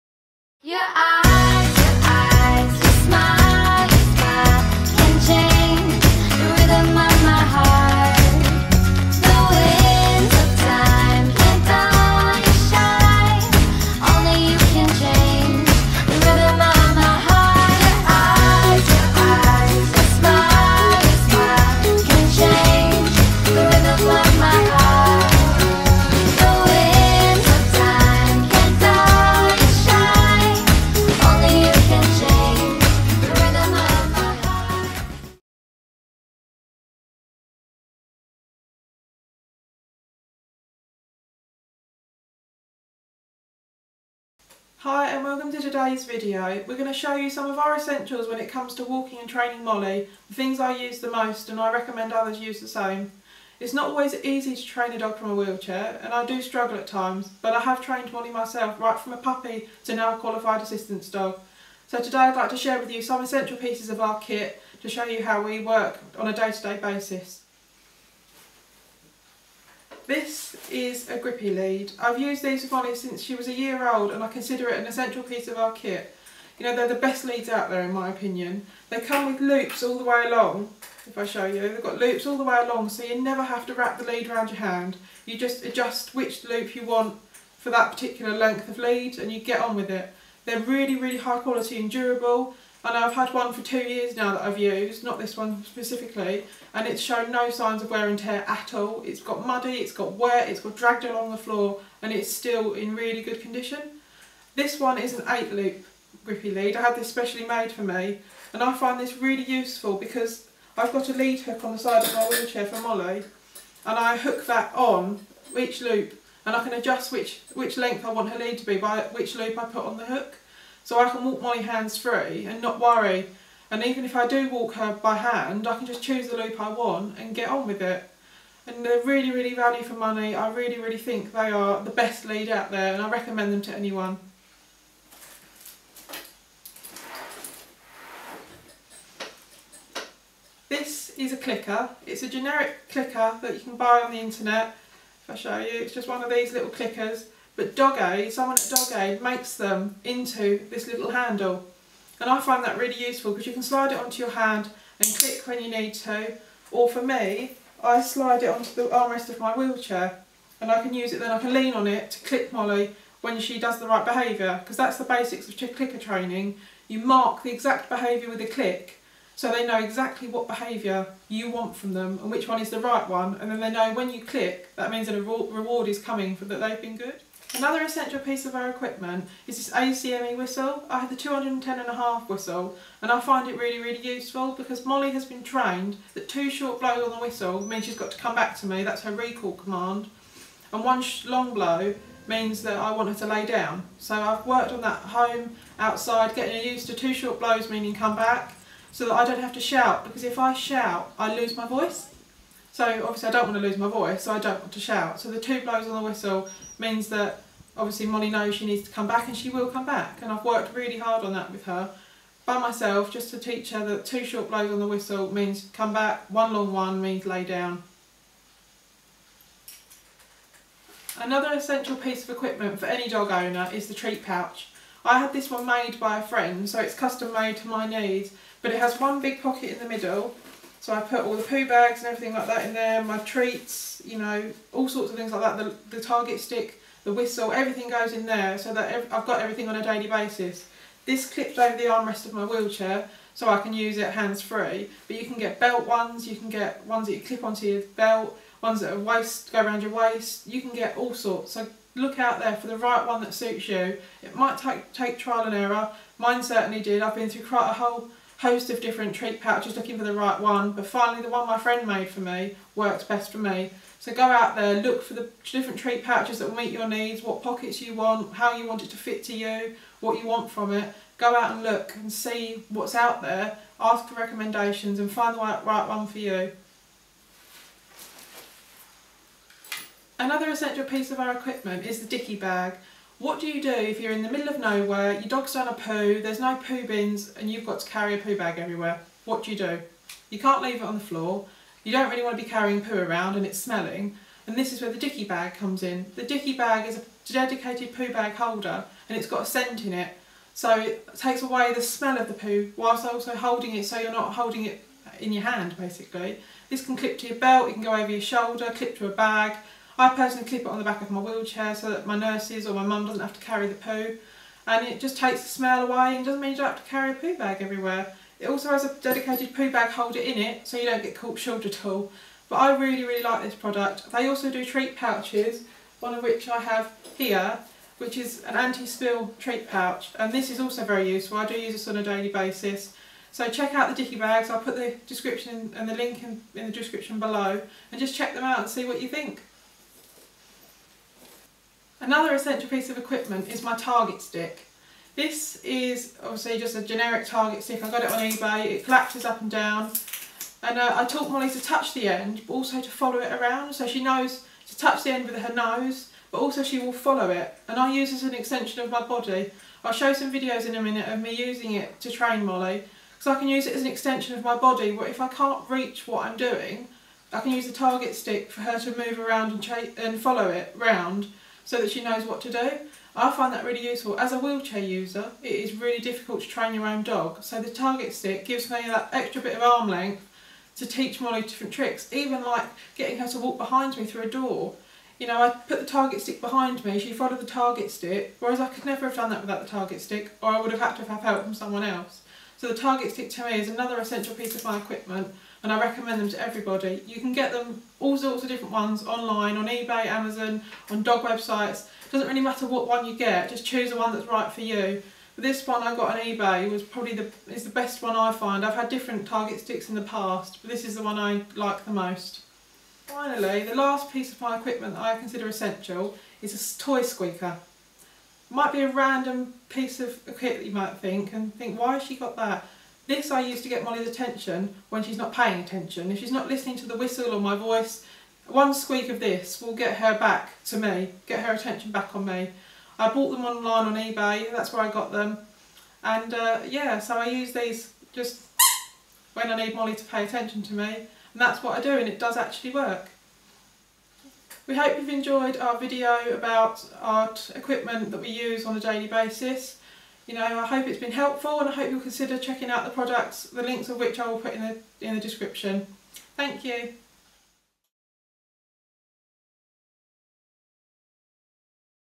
Vlog #3 - Essential Equipment - Molly, Dog With A Blog
A vlog about equipment I consider essential when walking and training Molly. There's far more we use on a daily basis but in the interest of not making it too long I have kept it to only some of the bare essentials.